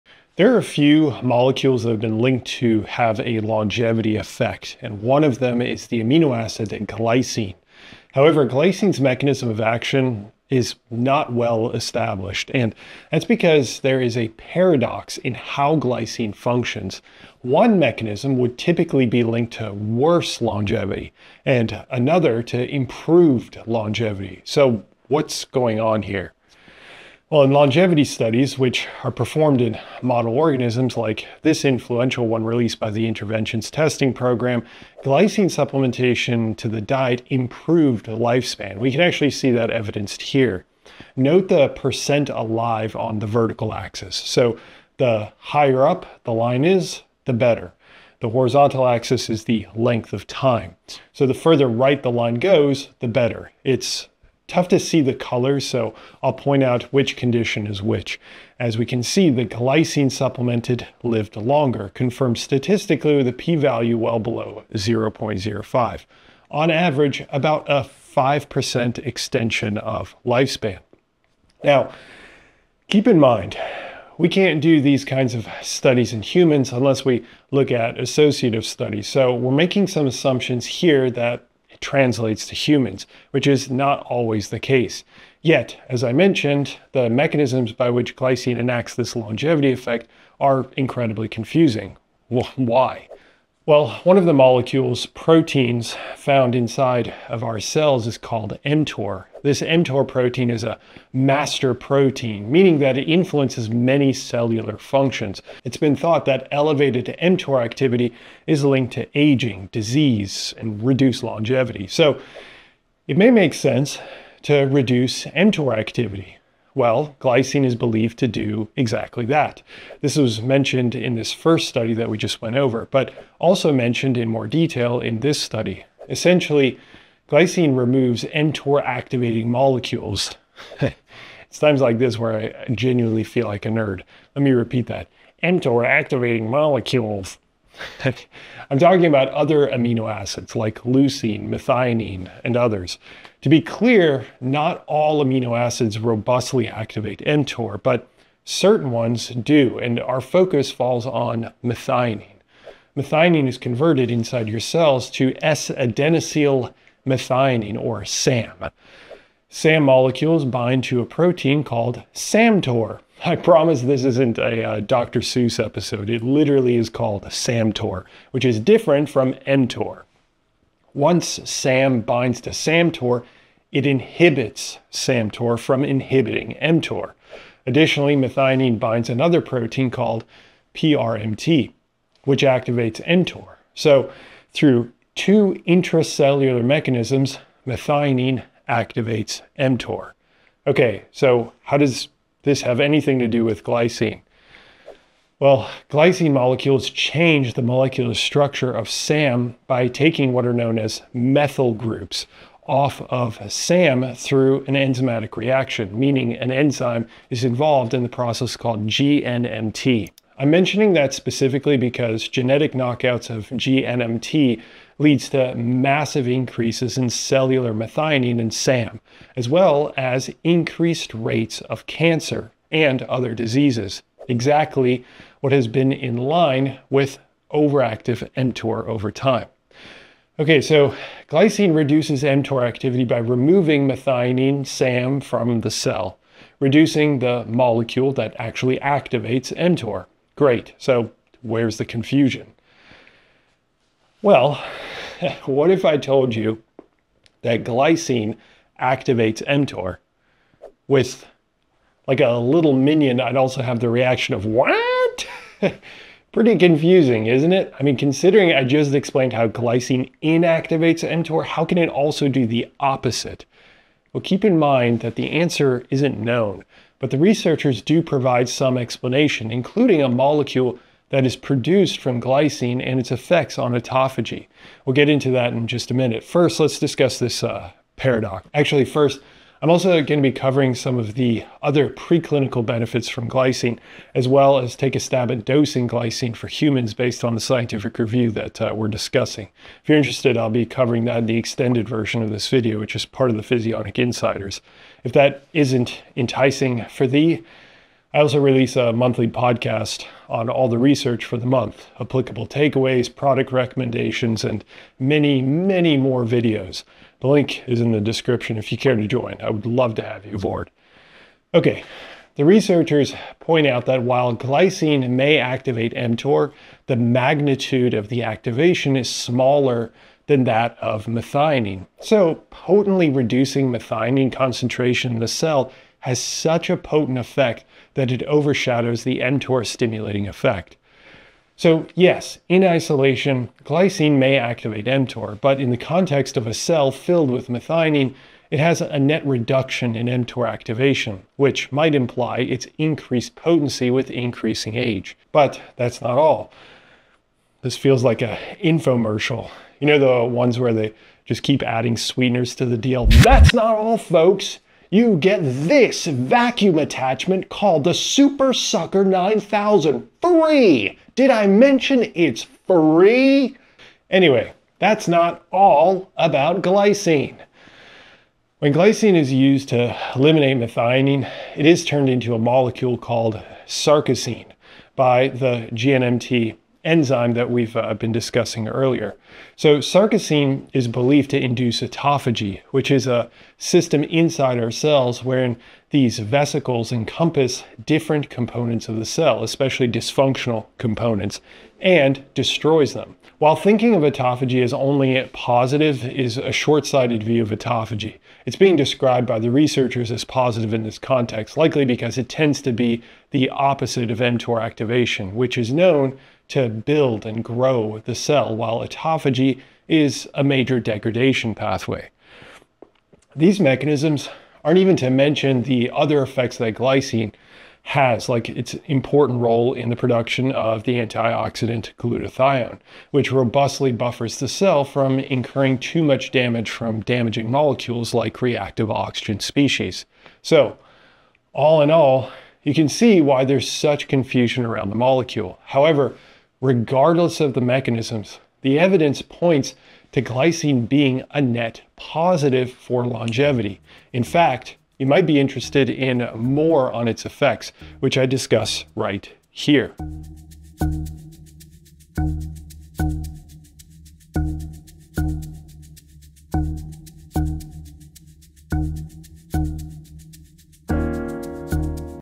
The Confusion surrounding Glycine’s Longevity Effect
References
[Study 297] Miller RA, Harrison DE, Astle CM, et al. Glycine supplementation extends lifespan of male and female mice. Aging Cell. 2019;18(3):e12953. doi:10.1111/acel.12953

[Study 348] Johnson AA, Cuellar TL. Glycine and aging: Evidence and mechanisms. Ageing Res Rev. 2023;87:101922. doi:10.1016/j.arr.2023.101922

Industry Funding: Tally Health / Potential Conflicts of Interest - All researchers are employees of research